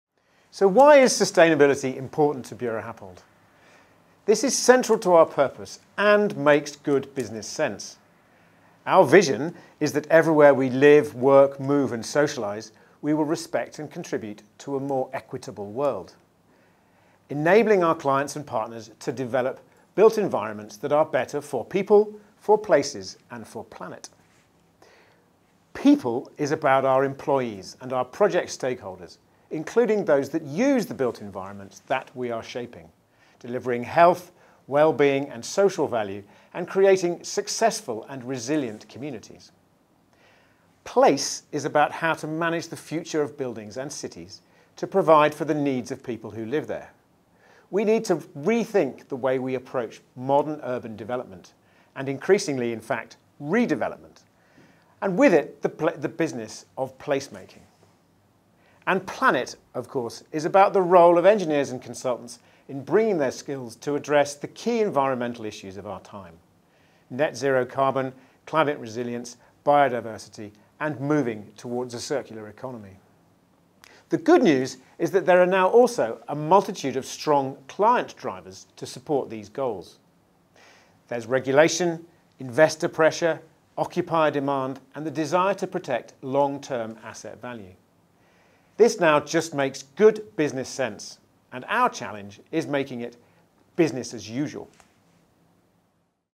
Global Sustainability Report 2022 with Oliver Plunkett, CEO
Sustainability is central to our purpose. Not only is tackling climate change critical for the future of us all, it makes good business sense.

In this video, CEO Oliver Plunkett explains why sustainability is important to Buro Happold. He considers how engineers and consultants use their skills to address the key environmental issues of our time: net zero carbon, climate resilience, biodiversity and the move towards a circular economy.

Watch Oliver discuss the importance of taking action to develop a built environment that is better for people, places and planet